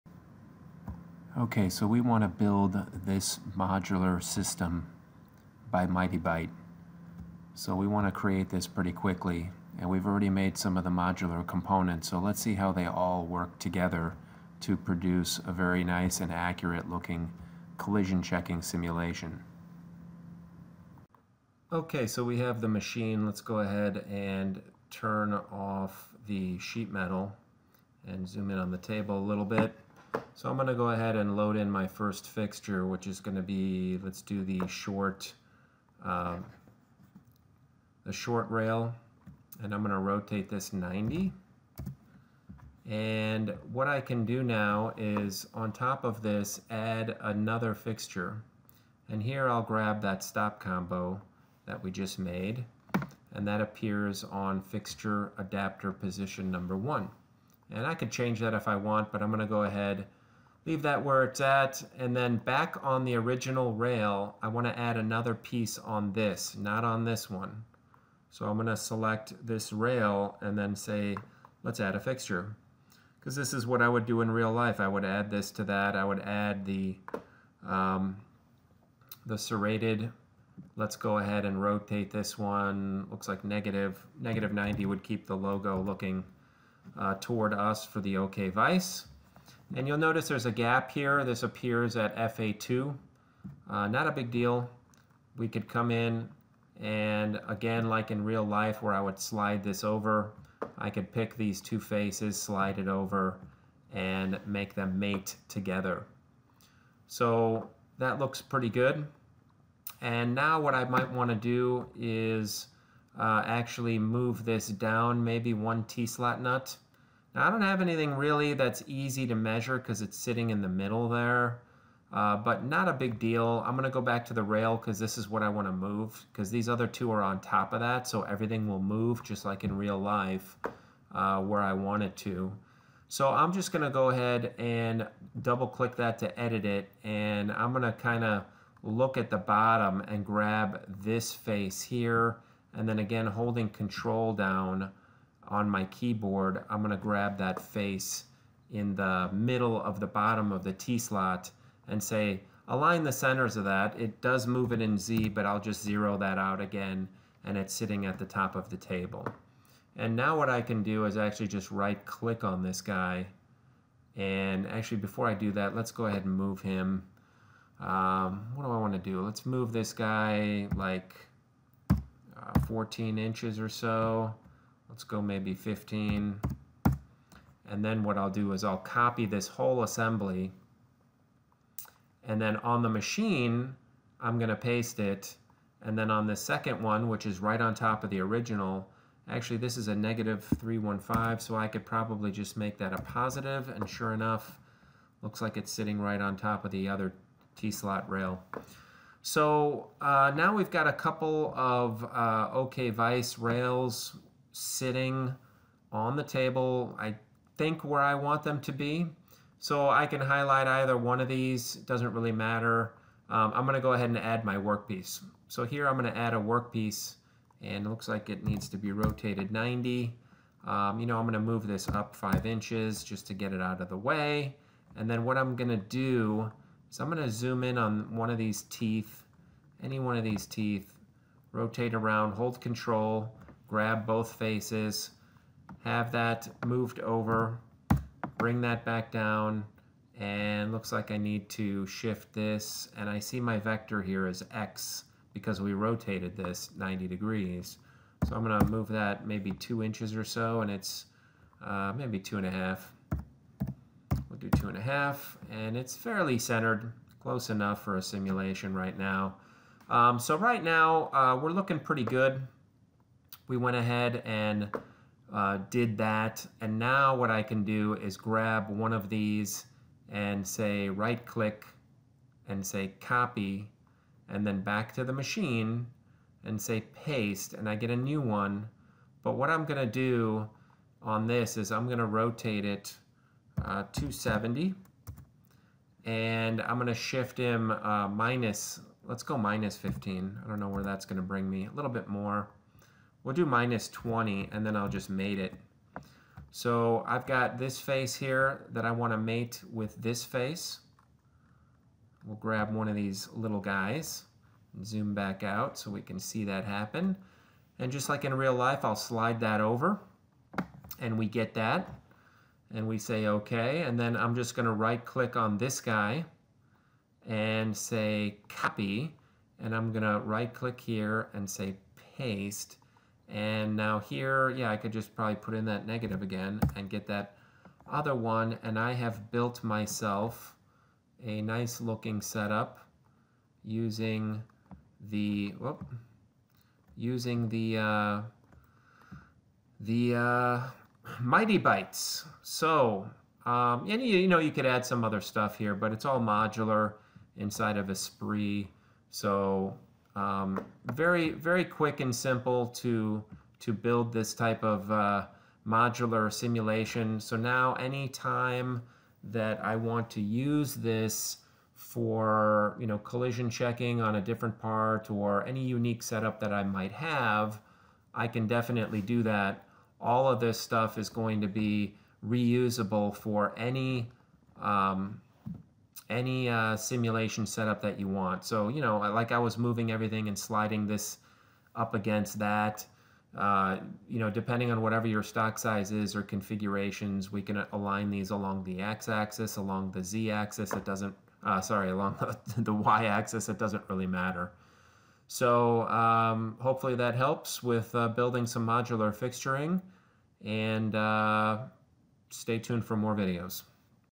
Using and Assembling The Mitee Bite Multi-Rail Components in ESPRIT
In this video, you will learn how to build and assemble the Mitee Bite multi-rail components modular system in ESPRIT.